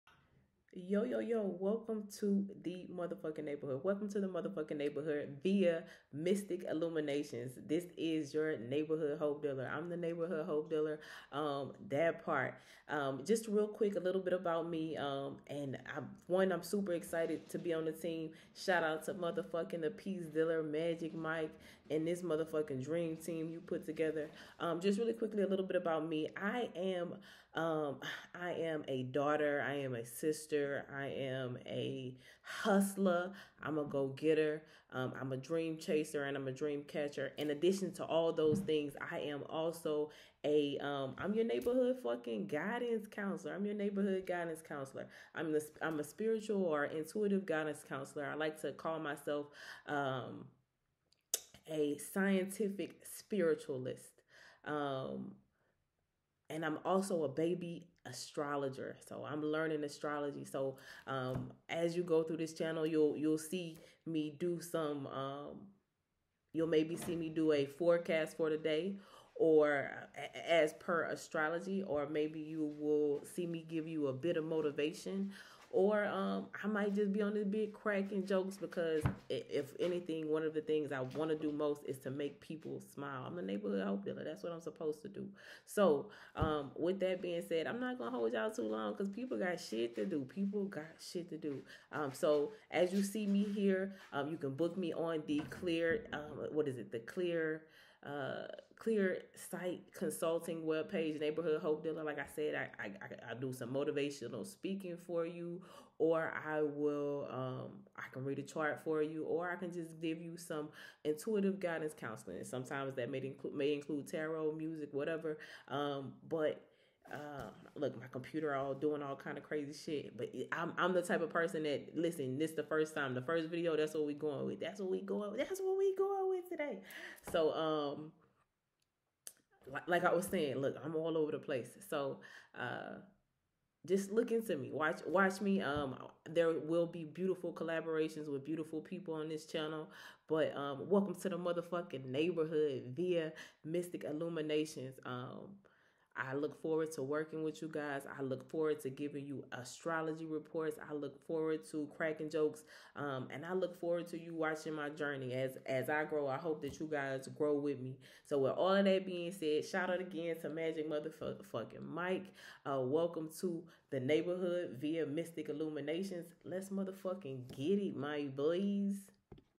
Your Neighboorhood Intuitive Guidance Counselor
What's popping world! Welcome to the Neighboorhood via Mystical Illuminations. Get comfortable and let's get to know each other. I'm Leand but I go by Neighboorhood Hope Dealer. I read cards and charts and I offer guidance where requested but I'm sooo much more and magical than that. I'm an everyday human with interesting hobbies lol. Im a multifaceted enigma and I'm ok with that, and you should be too! Let's change the world folks! :)

I'll be updating and restructuring my pricing and services so bear with me pretty please. I want to make sure I give you all the bangs I can for your buck.. nohomo lol

Welcome to the Neighboorhood via Mystical illumination world!

-NHD